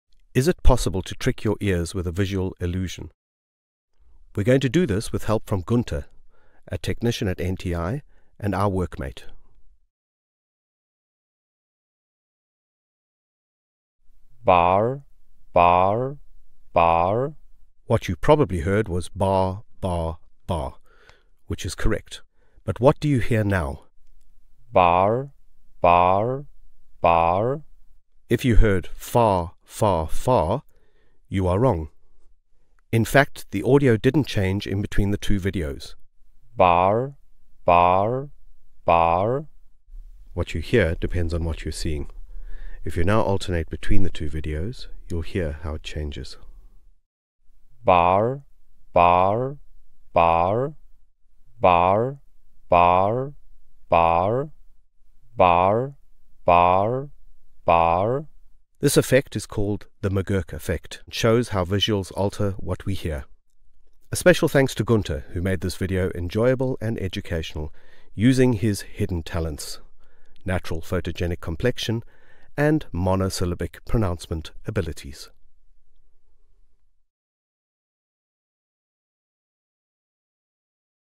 神奇的麦格克效应
视觉是否会影响听觉？要不要试试？